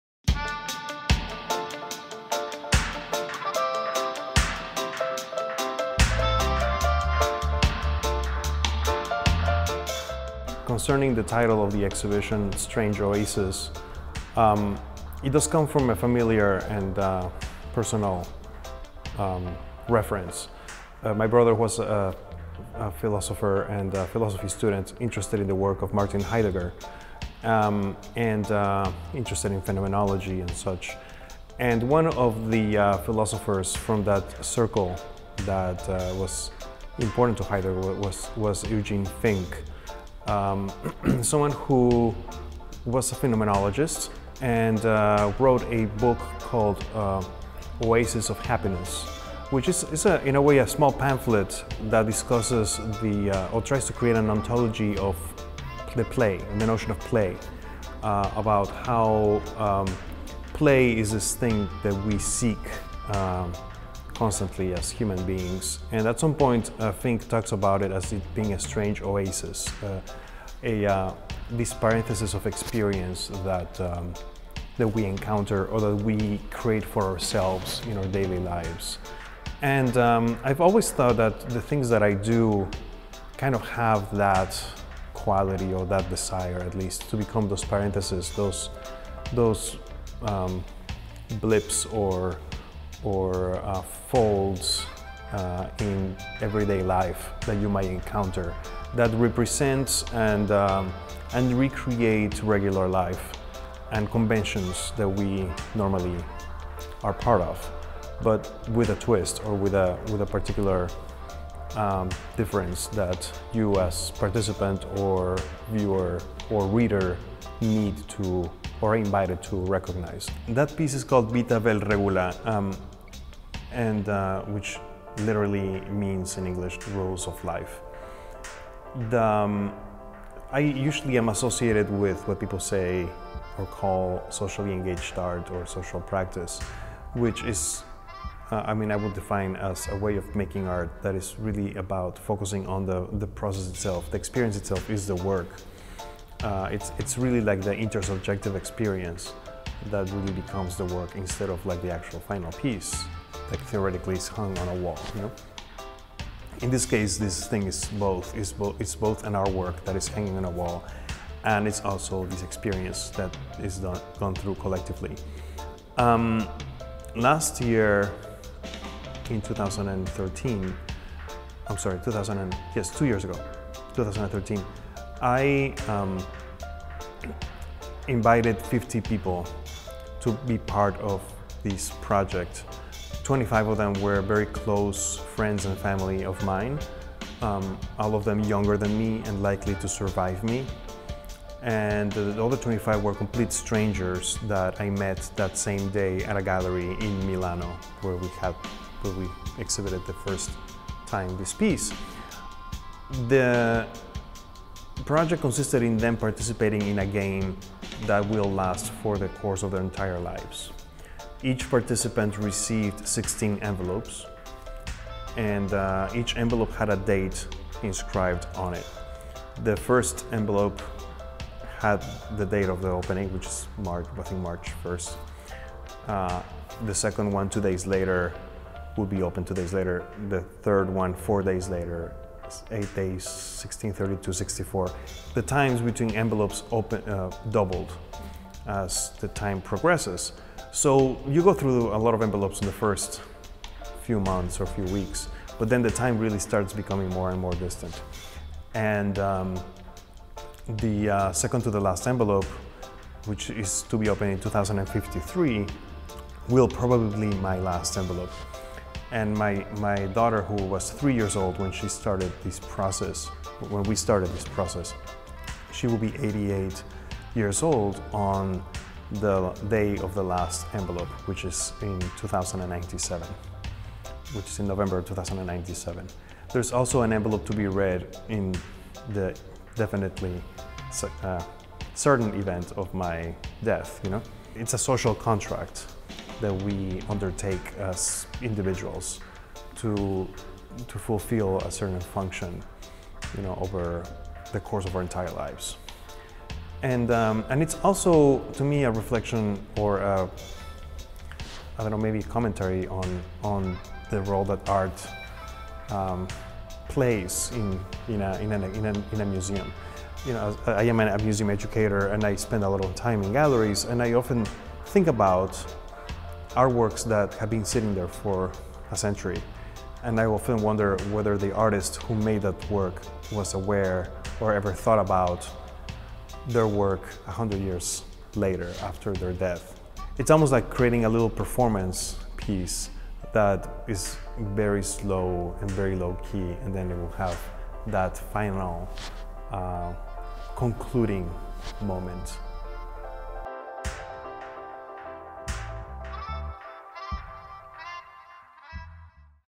Pablo Helguera defines the notion of ‘play’ in “Strange Oasis”
Pablo Helguera, interdisciplinary artist, sits down with ArtInfo to talk about his new installation with Kent Fine Art, Strange Oasis.